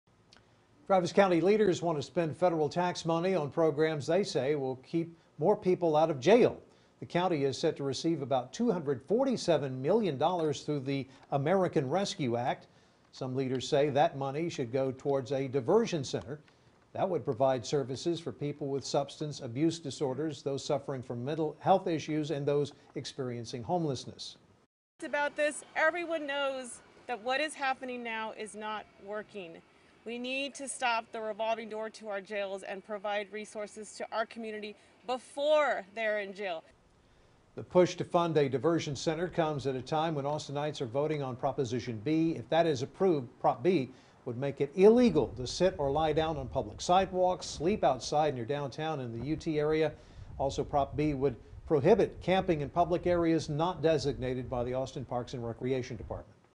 Travis County leaders push for 'diversion center' to get people help they need
Travis County leaders want to create a service to help people to get support and help before they interact with police, and they want to use federal dollars to make it happen.